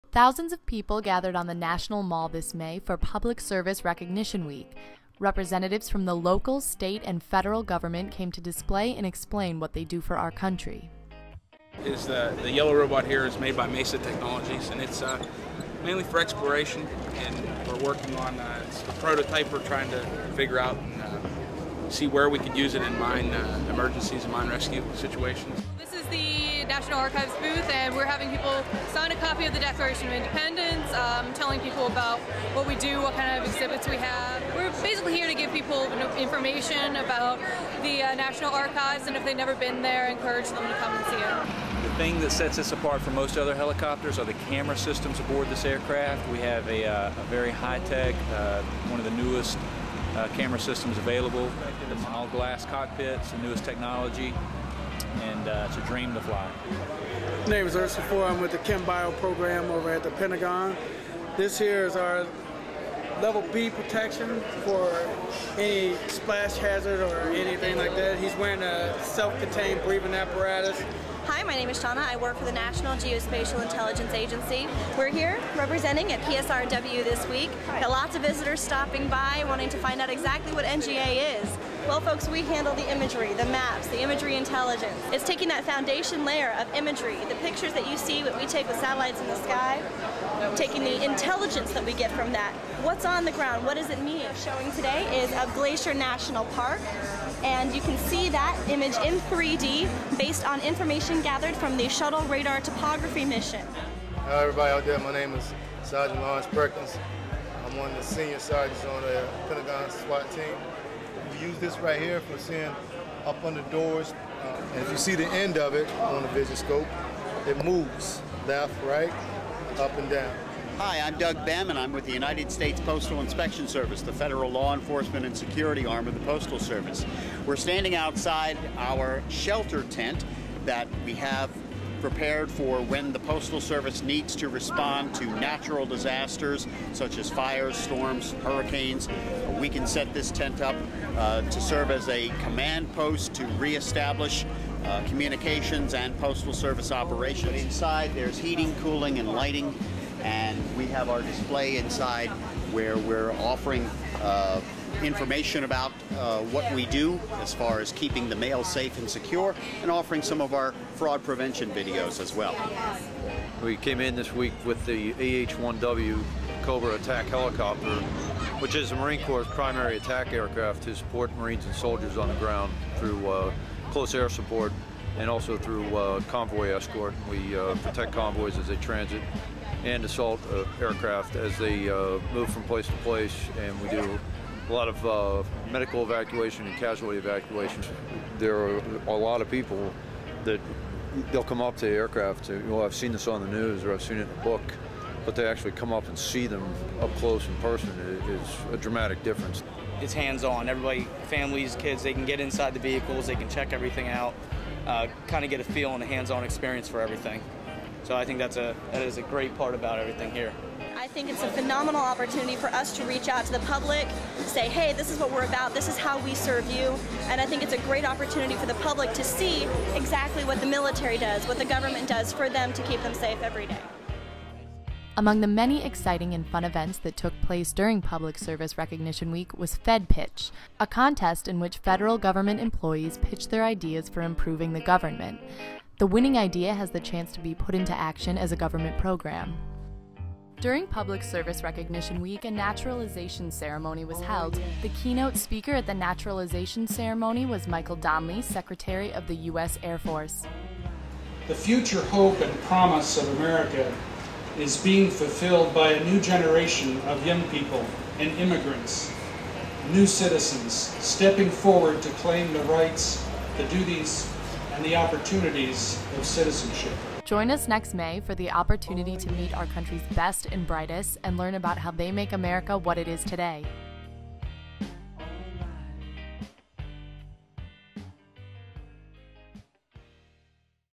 Public Service Recognition Week 2008
Celebrated since 1985 the first Monday through Sunday in May, Public Service Recognition Week (PSRW) is a time set aside to honor the men and women who serve our nation as federal, state, county and local government employees and ensure that our government is the best in the world. Throughout the nation and around the world, public employees use the week to educate citizens about the work that they do and why they have chosen public service careers, as well as the many ways government services make life better for all of us. The capstone celebration is held every year on the National Mall, with more than 100 government offices and agencies, nonprofit organizations and private companies sponsoring interactive and educational exhibits that showcase the innovative and quality work performed by public employees. This year, the Partnership and the Public Employees Roundtable will host the National Mall event May 7-10, 2009 between 4th and 7th Sts.